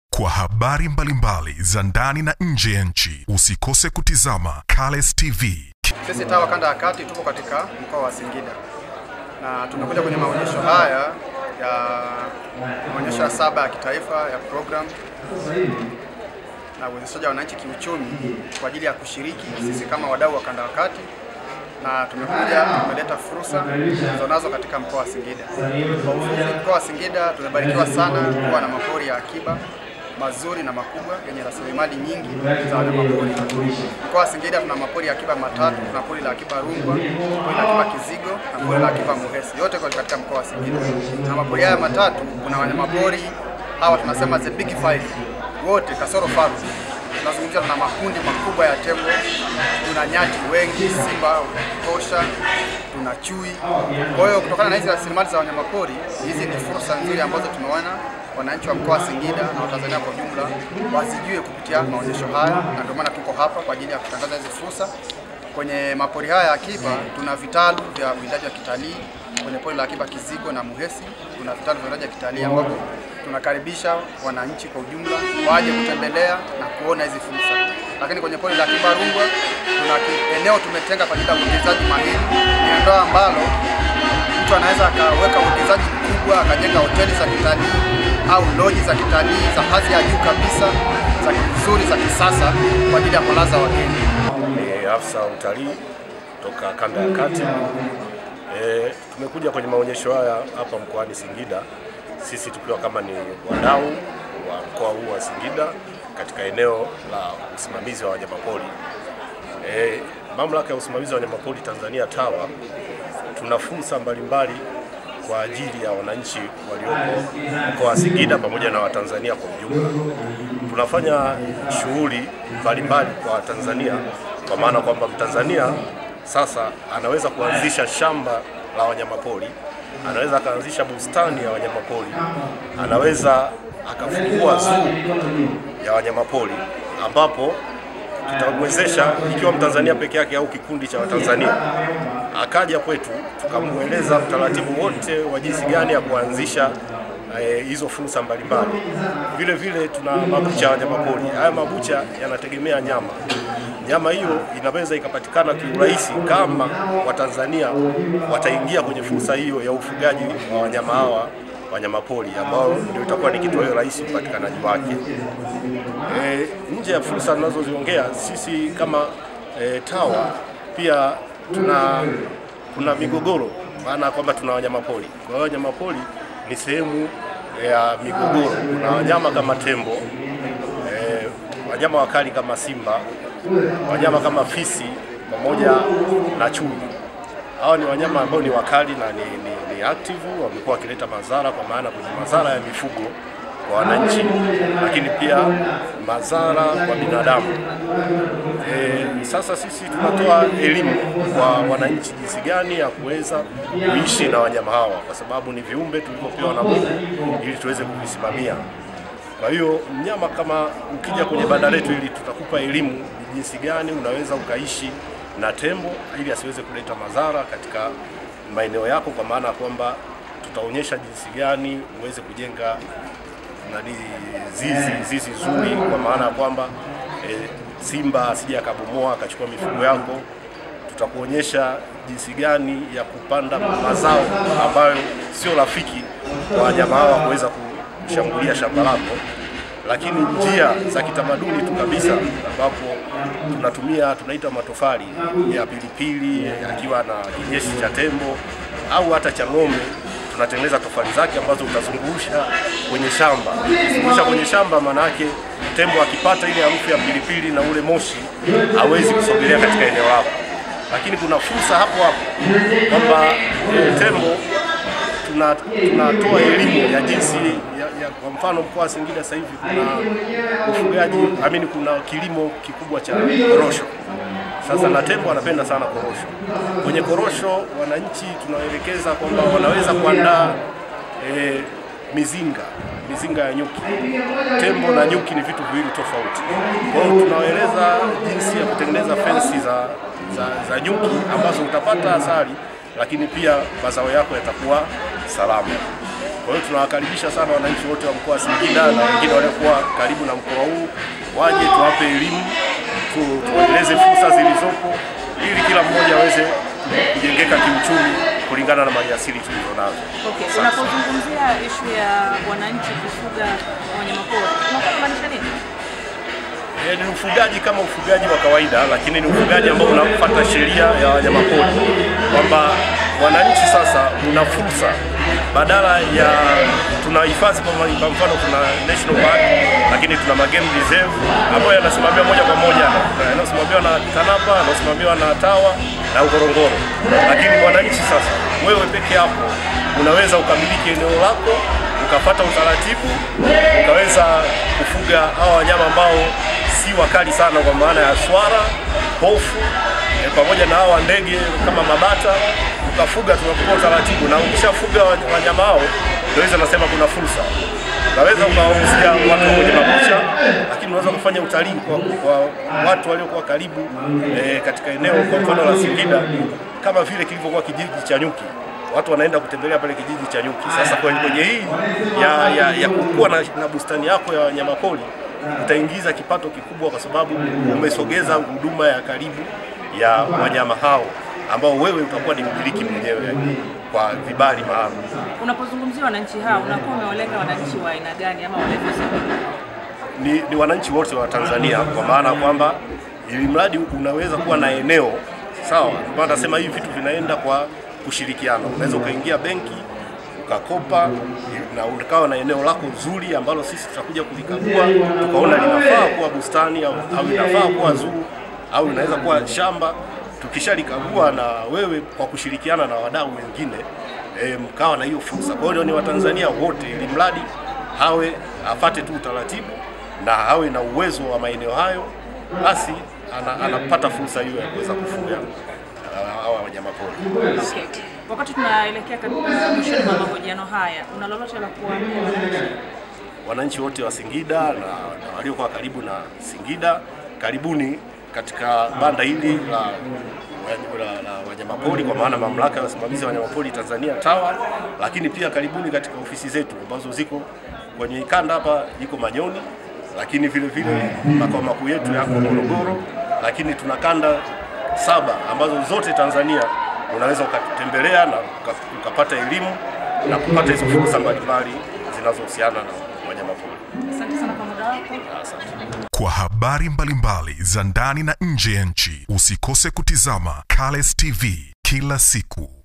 TAWA WATOA ELIMU JUU YA WANYAMA PORI.
Mamlaka ya wanayama pori nchini kanda ya kati TAWA wameendelea kutoa elimu juu ya uifadhi wa wanyama pori na ufugaji wake,akiongea na waandishi wa habari Afisa uifadhi wa wanayama pori kanda ya kati Sylvester Mdoe amesema kuwa Tawa wamekuwa wakitoa vibari mbalimbali juu wa uwindaji na ufugaji wanyama pori wapole na sio wakali ili kukalibisha wawekezaji kuja kuwekeza katika mbuga za wanyama hao zilizopo nchini.